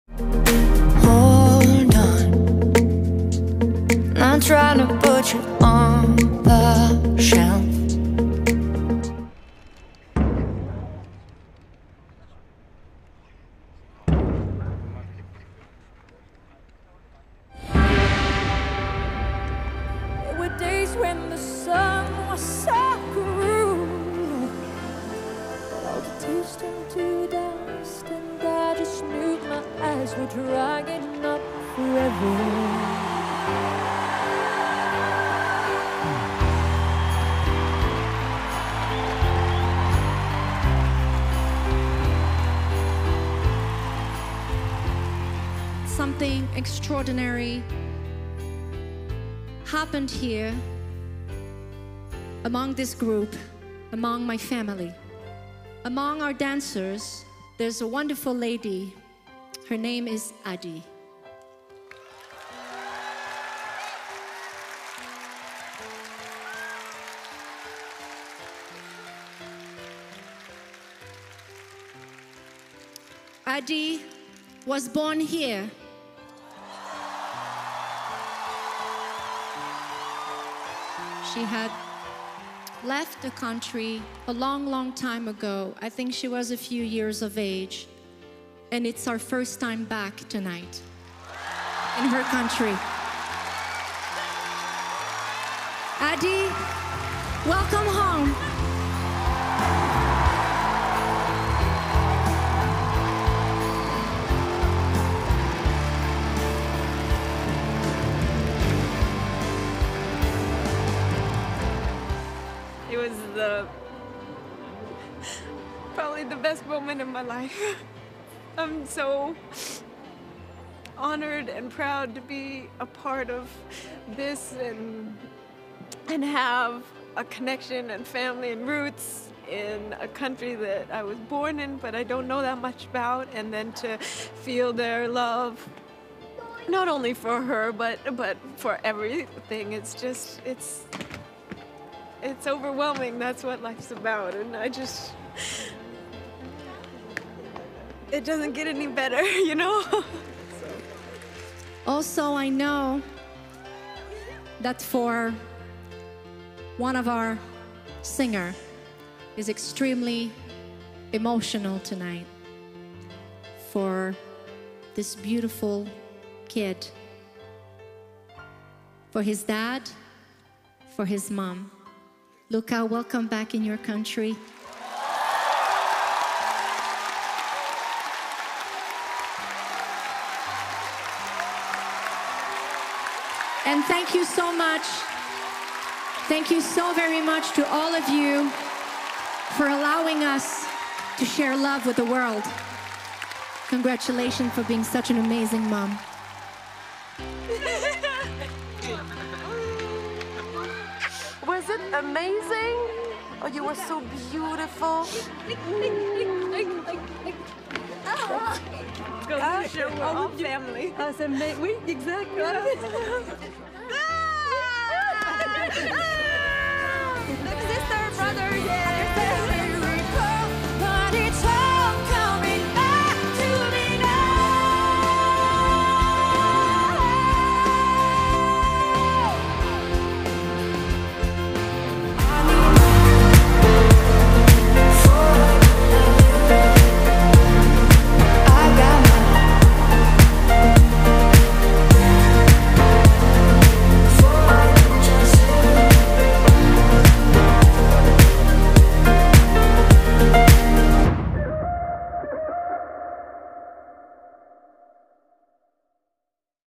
Céline Dion Introduces South Korean Backup Dancer To Audience During Seoul Concert (2008)
Hashtag(s)